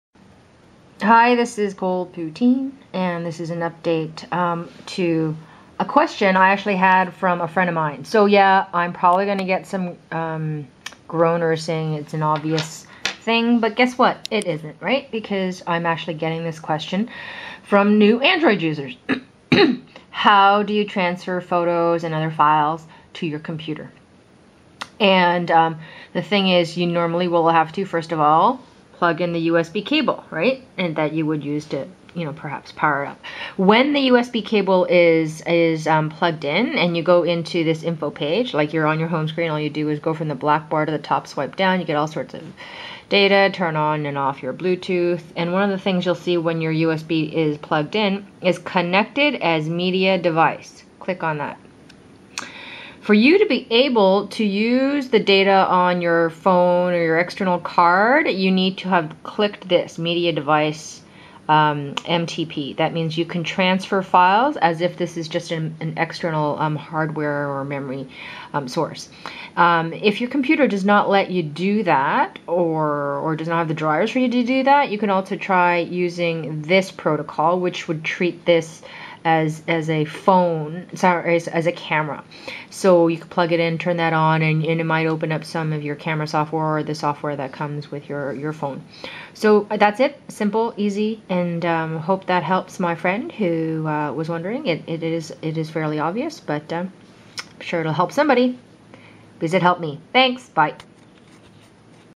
How to transfer photos from your Samsung Galaxy Note II via USB
This one is terribly obvious, well, once you figure it out, as always.